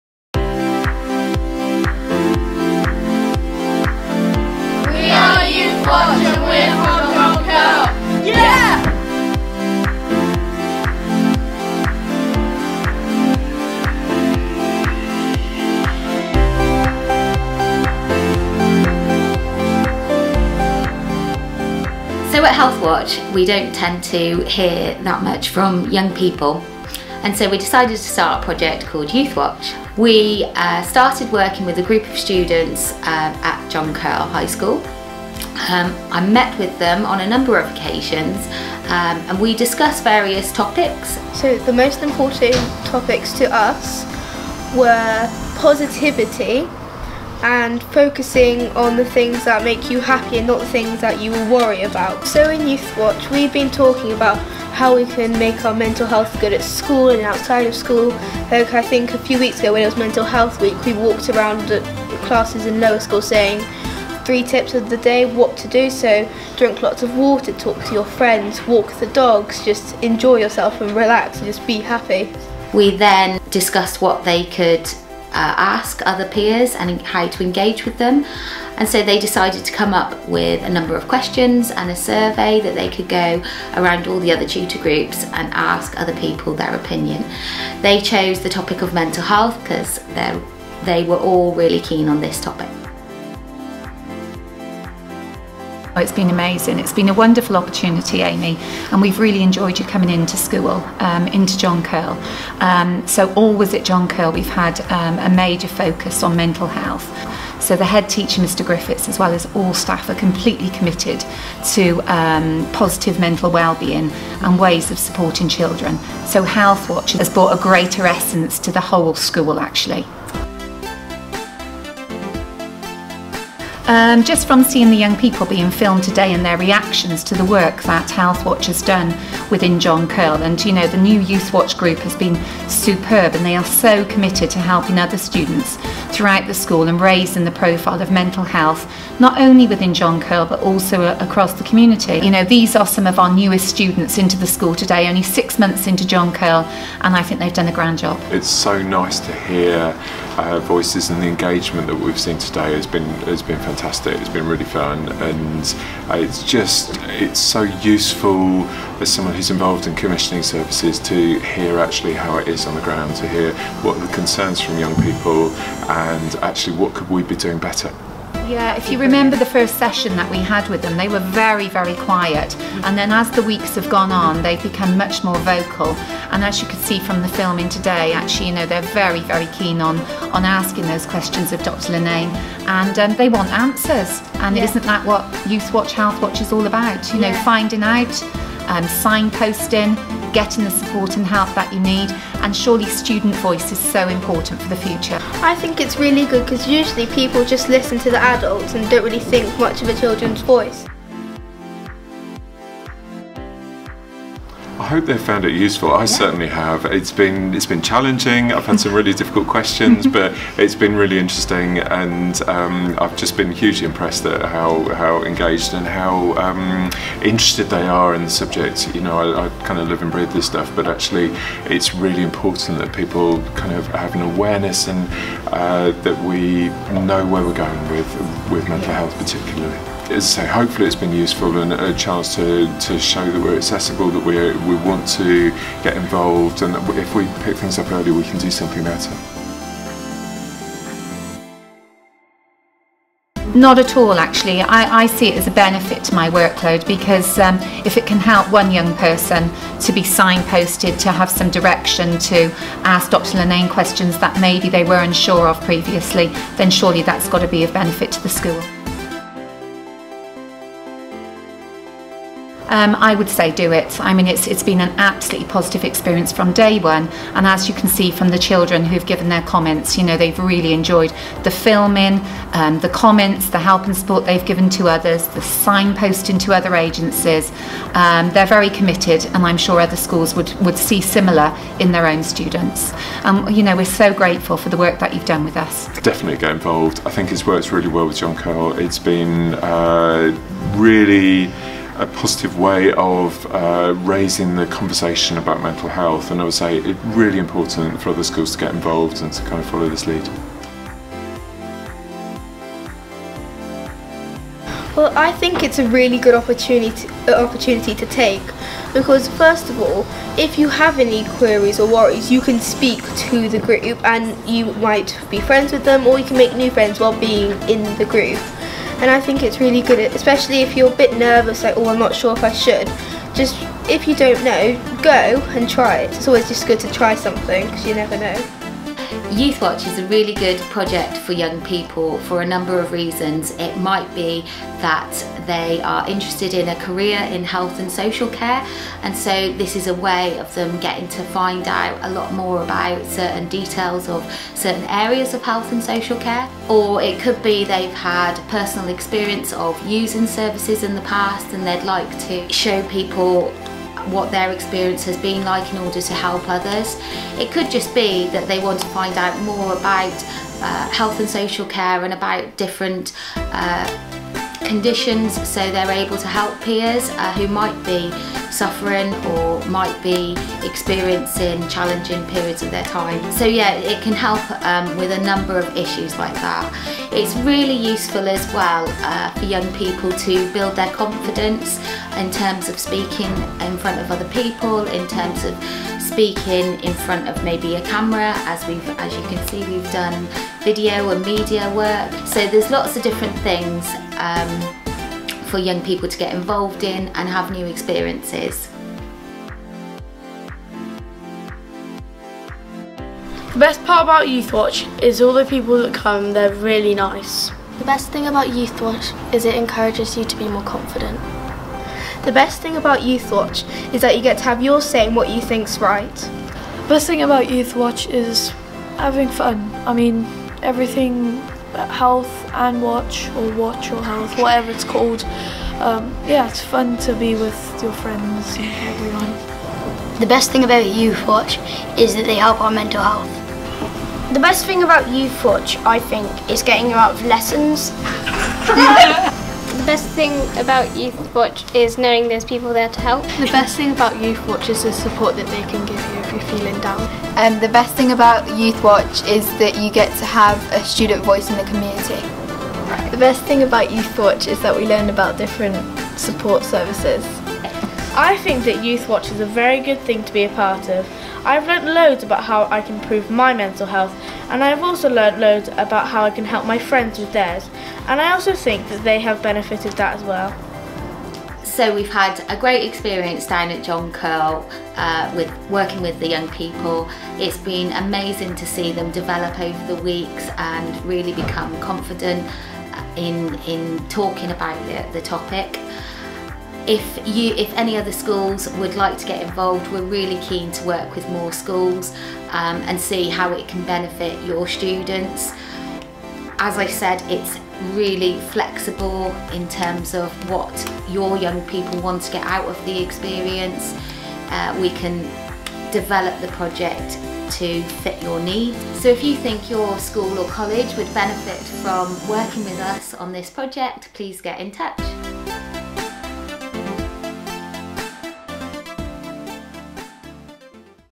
Youthwatch Herefordshire Video
A short video explaining the work undertaken by John Kyrle High School students through Healthwatch's Youthwatch scheme.